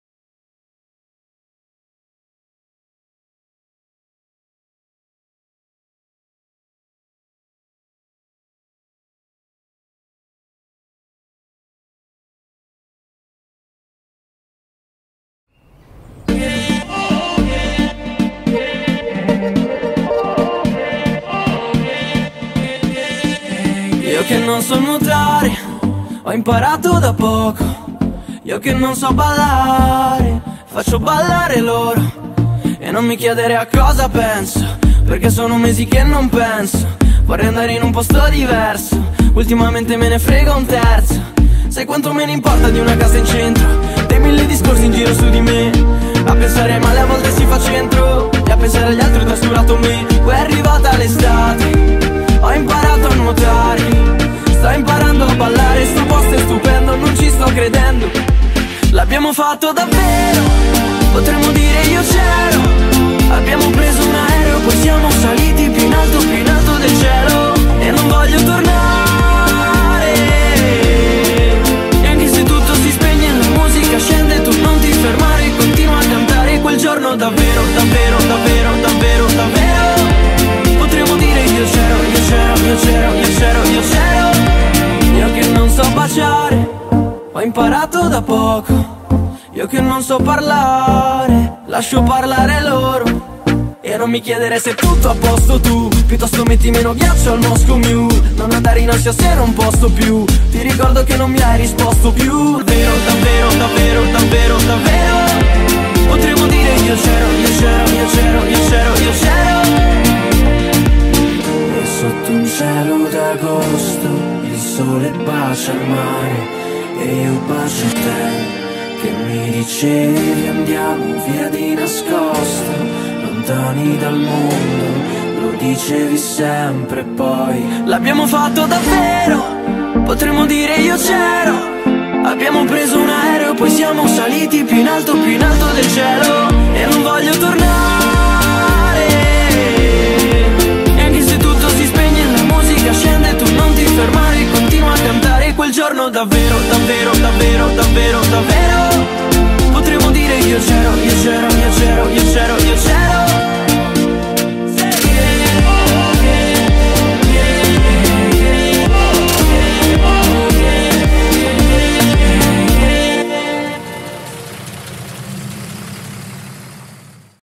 ESSERI SPECIALI #6 REY BEACH CLUB 2018
Ecco a voi il saluto degli ospiti del Rey Beach Club estate 2018.